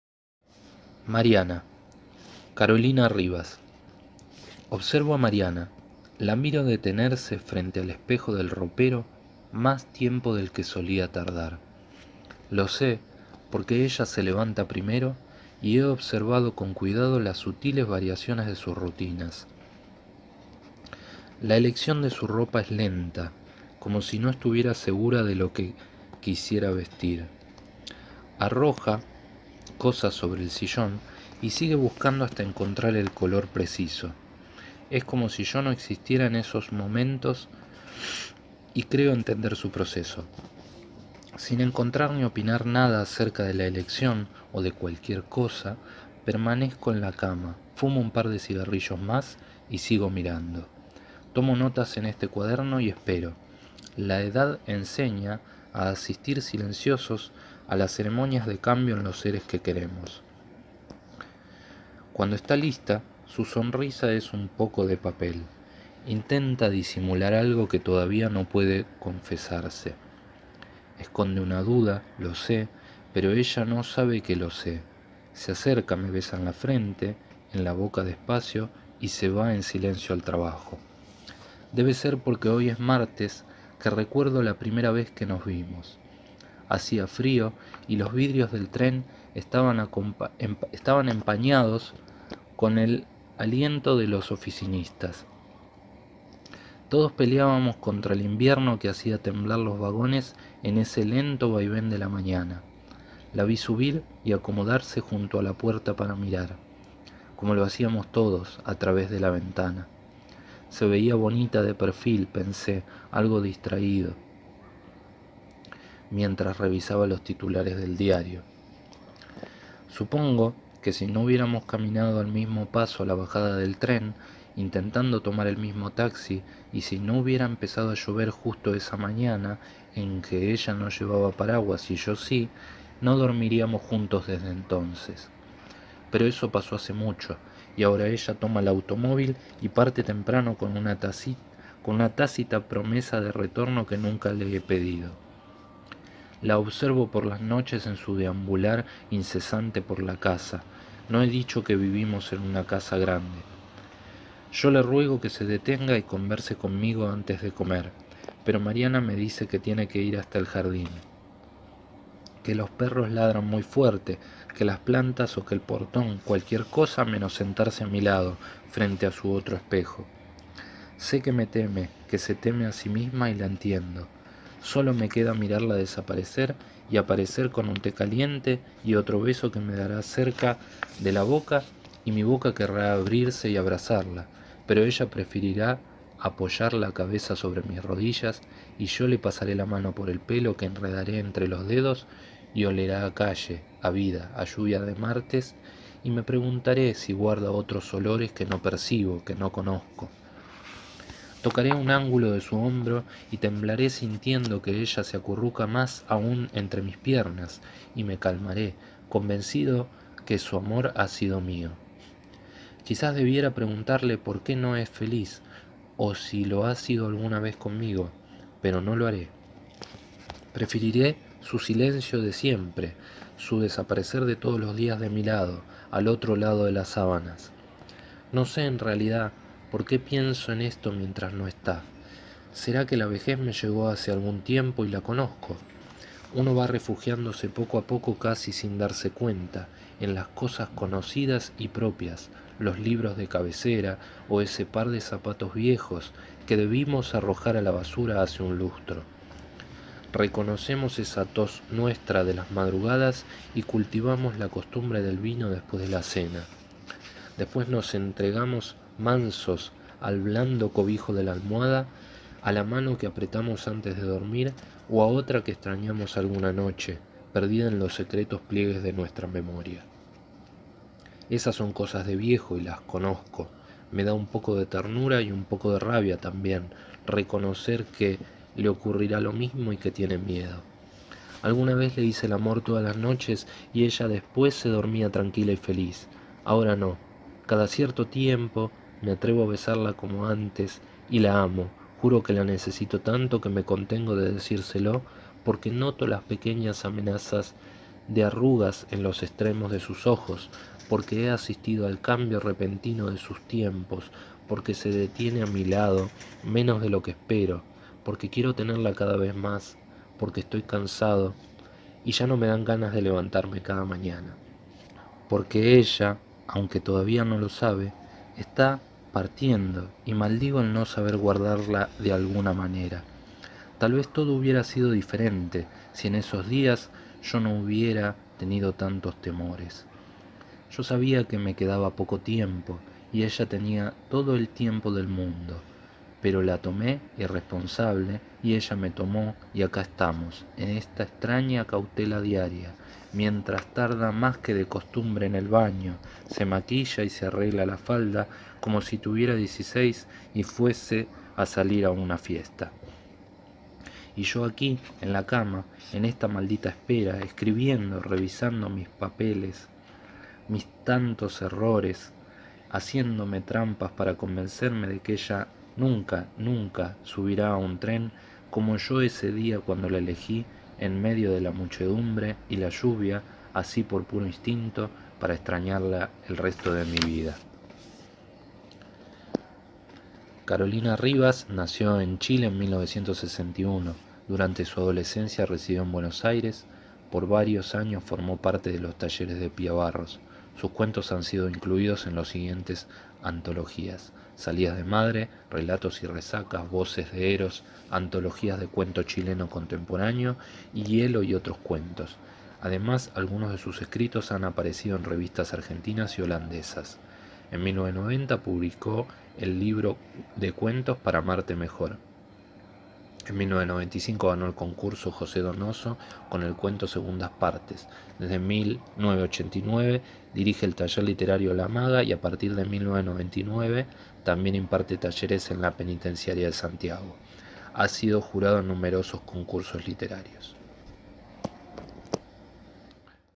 Mariana - Carolina Rivas
Tomado de la edición "Palabras escritas para vos" durante el gobierno de Cristina Fernández de Kirchner, actual Vicepresidenta de la Nación.
"Este libro no hubiera sido posible, sin la colaboración de autores y editoriales que cedieron los derechos para esta obra" -
Ministros y Secretarios de educación durante el gobierno, en el año 2008 Prof Alberto Sileoni  y Prof Juan Carlos Tedesco